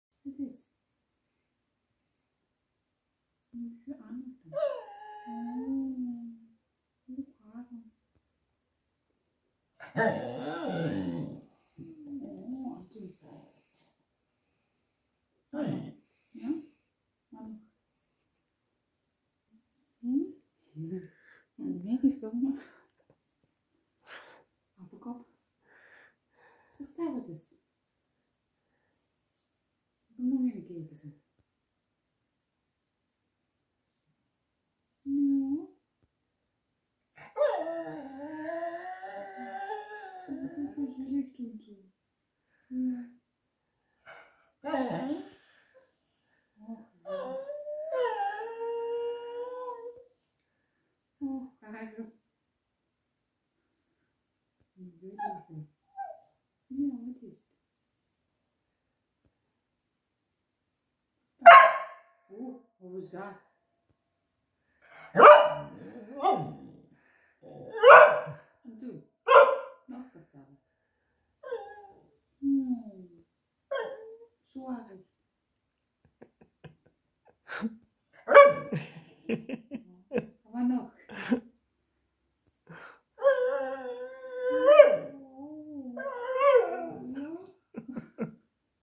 Does your beagle talk to you too?
A sweet pathetic beagle who has a conversation with you.

My young sweet beagle Rexa would like to have a snack but is not getting it. That is why she is now going to act pathetic and complain in the kitchen. While she tries to enter into a conversation why she deserves a snack. But will that work?